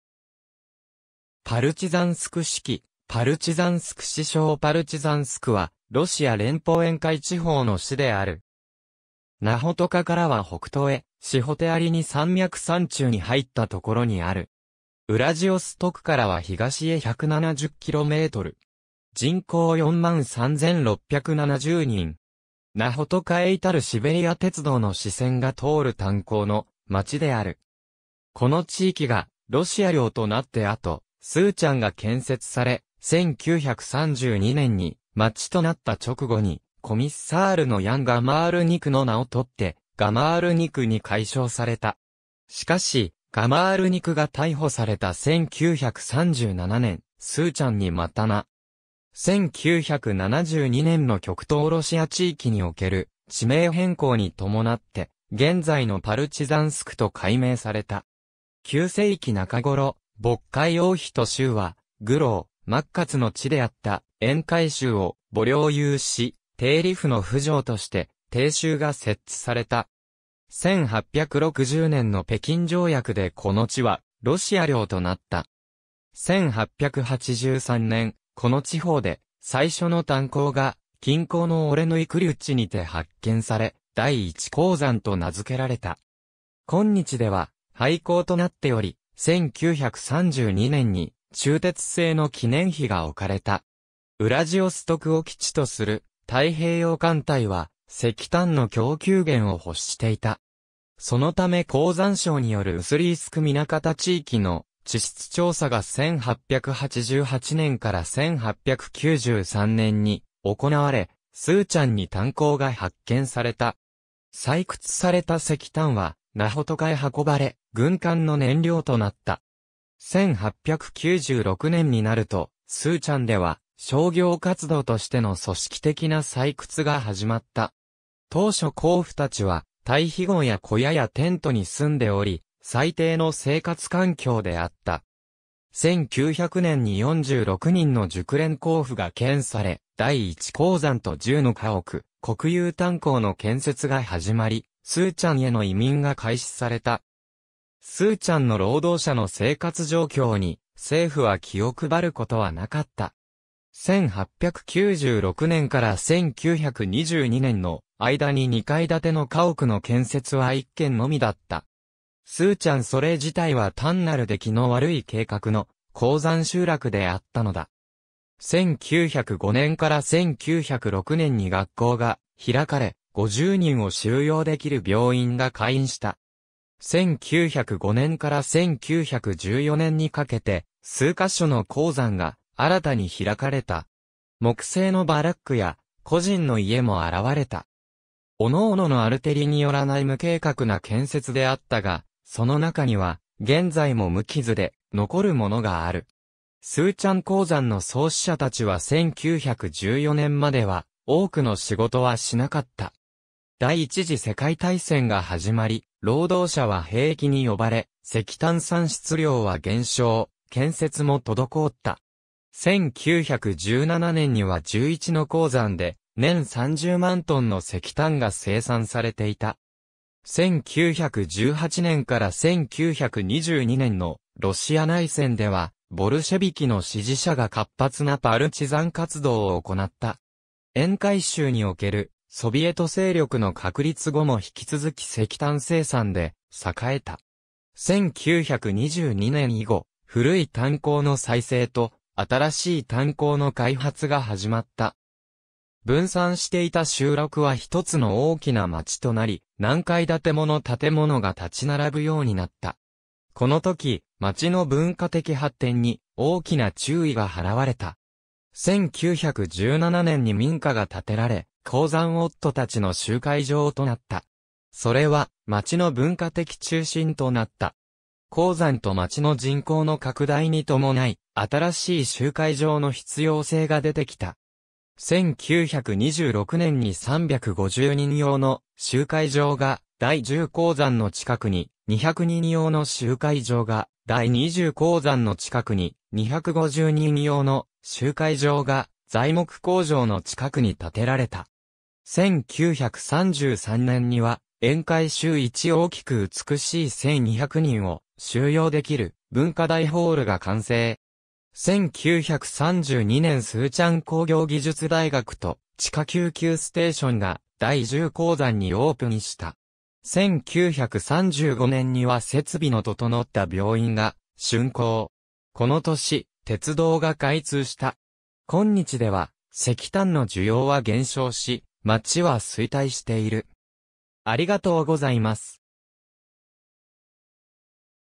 パルチザンスク
パルチザンスク市旗 パルチザンスク市章 パルチザンスク（ロシア語: Партизанск）は、ロシア連邦沿海地方の市である。
ナホトカからは北東へシホテアリニ山脈山中に入ったところにある。
ウラジオストクからは東へ170キロメートル。
ナホトカへ至るシベリア鉄道の支線が通る炭鉱の町である。
この地域がロシア領となって後、スーチャン（Сучан、蘇城）が建設され、1932年に町となった直後に、コミッサールのヤン・ガマールニクの名をとってガマールニク（Гамарник）に改称された。
しかしガマールニクが逮捕された1937年、スーチャンに復名。
1972年の極東ロシア地域における地名変更に伴って、現在のパルチザンスクと改名された。
9世紀中頃、渤海大仁秀は虞婁靺鞨の地であった沿海州をぼ領有し、定理府の府城として定州が設置された。
1860年の北京条約でこの地はロシア領となった。
1883年、この地方で最初の炭鉱が近郊のオレヌイ・クリュチにて発見され第1鉱山と名づけられた。
今日では廃坑となっており、1932年に鋳鉄製の記念碑が置かれた。
ウラジオストクを基地とする太平洋艦隊は石炭の供給源を欲していた。
そのため鉱山省によるウスリースク南方地域の地質調査が1888年～1893年に行われ、スーチャンに炭鉱が発見された。
採掘された石炭はナホトカへ運ばれ、軍艦の燃料となった。
1896年になるとスーチャンでは商業活動としての組織的な採掘が始まった。
当初鉱夫たちは待避壕や小屋やテントに住んでおり、最低の生活環境であった。
1900年に46人の熟練鉱夫が遣され、第1鉱山と10の家屋、国有炭鉱の建設が始まり、スーチャンへの移民が開始された。
スーチャンの労働者の生活状況に政府は気を配ることはなかった。
1896年から1922年の間に2階建ての家屋の建設は1件のみだった。
スーチャンそれ自体は単なる出来の悪い計画の鉱山集落であったのだ。
1905年～1906年に学校が開かれ、50人を収容できる病院が開院した。
1905年から1914年にかけて数箇所の鉱山が新たに開かれた。
木製のバラックや個人の家も現れた。
各々のアルテリによらない無計画な建設であったが、その中には現在も無傷で残るものがある。
スーチャン鉱山の創始者たちは1914年までは多くの仕事はしなかった。
第一次世界大戦が始まり、労働者は兵役に呼ばれ、石炭産出量は減少、建設も滞った。
1917年には11の鉱山で年300,000tの石炭が生産されていた。
1918年～1922年のロシア内戦ではボルシェヴィキの支持者が活発なパルチザン活動を行った。
沿海州におけるソヴィエト勢力の確立後も引き続き石炭生産で栄えた。
1922年以後、古い炭鉱の再生と新しい炭鉱の開発が始まった。
分散していた集落は一つの大きな街となり、何階建てもの建物が立ち並ぶようになった。
この時、町の文化的発展に大きな注意が払われた。
1917年に民家が建てられ、鉱山夫たちの集会場となった。
それは町の文化的中心となった。
鉱山と町の人口の拡大に伴い、新しい集会場の必要性がでてきた。
1926年に350人用の集会場が第10鉱山の近くに、200人用の集会場が第20鉱山の近くに、250人用の集会場が材木工場の近くに建てられた。
1933年には沿海州一大きく美しい1200人を収容できる文化大ホールが完成。
1932年スーチャン鉱業技術大学と地下救急ステーションが第10鉱山にオープンした。
1935年には設備の整った病院が竣工。
この年、鉄道が開通した。
今日では石炭の需要は減少し、町は衰退している。